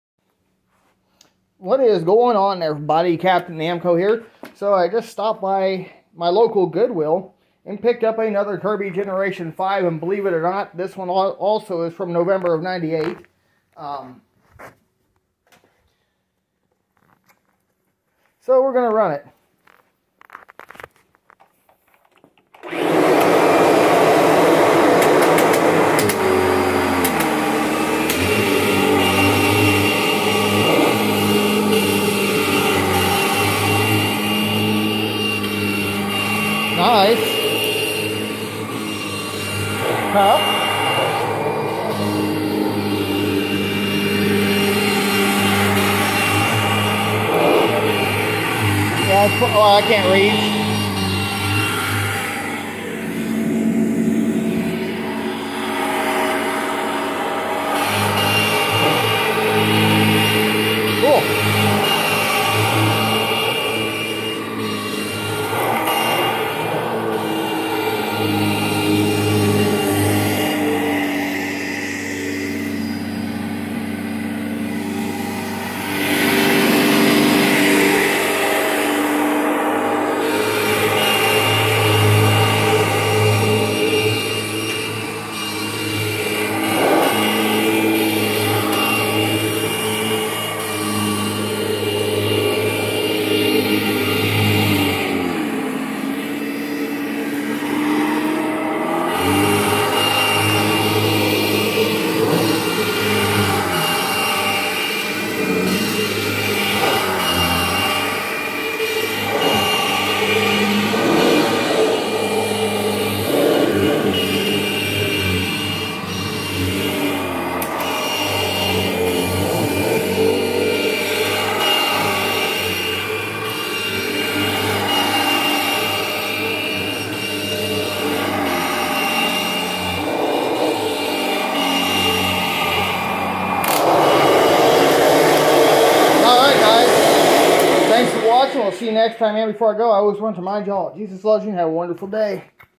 1998 Kirby Generation 5 Upright goodwill purchase
what is going on everyone captain namco here yesterday afternoon I went to goodwill and bought another Kirby upright vacuum and of course I inspected the fan the belt listened to it run
it was $29.99 USD (45.23 Aud)
+ $2.25 USD tax ($3.39 Aud)
for a total of $32.24 USD ($48.63 Aud)